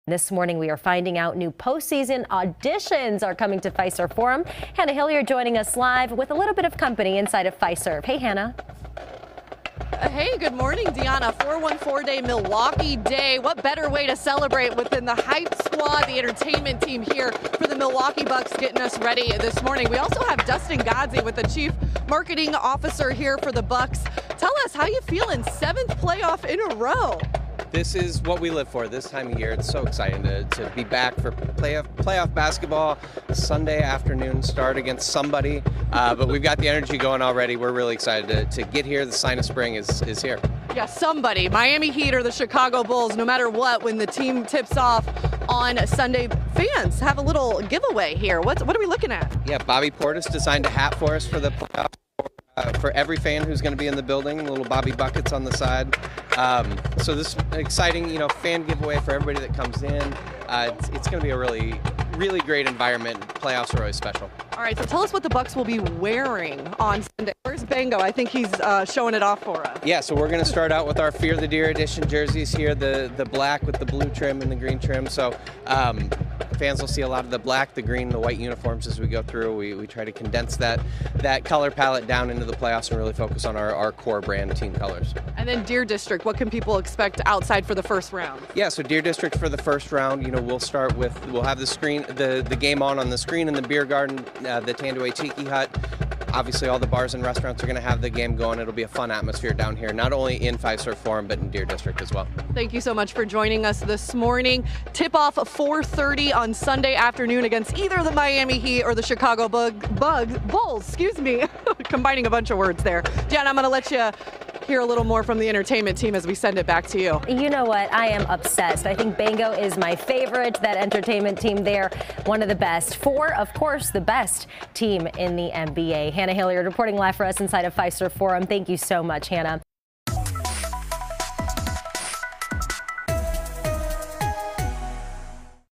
Milwaukee Bucks have energy going for playoffs
Bobby Portis designed a hat for fans for Game 1 on Sunday.